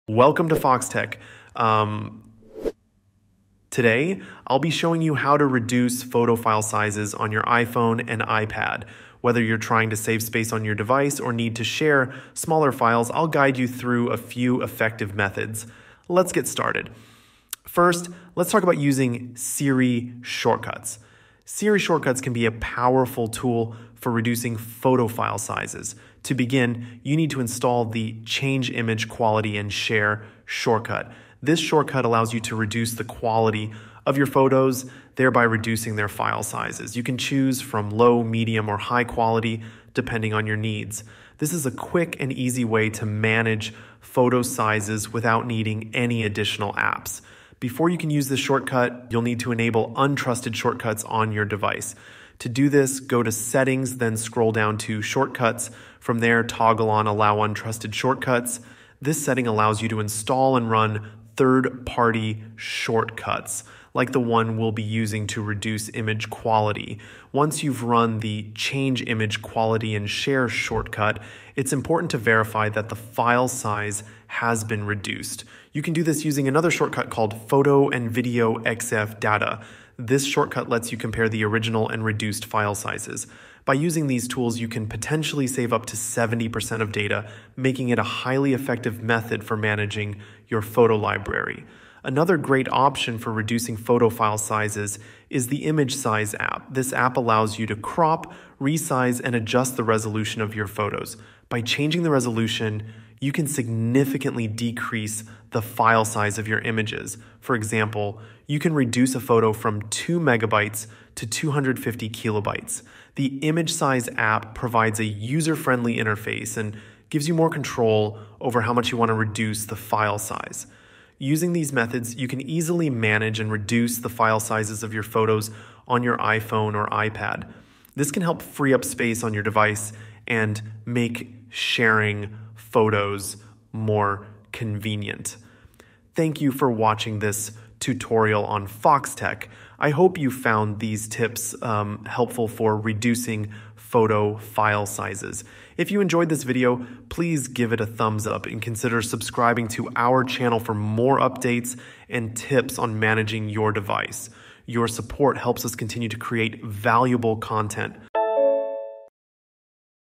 How to Reduce Photo File Size on iPhone and iPad (tutorial)
In this video, we show you how to reduce the file size of photos on your iPhone and iPad. Learn various methods and settings adjustments to compress images without losing quality. Follow our step-by-step guide to free up storage space and optimize your device’s performance. Watch now to master photo file size reduction on your iOS devices.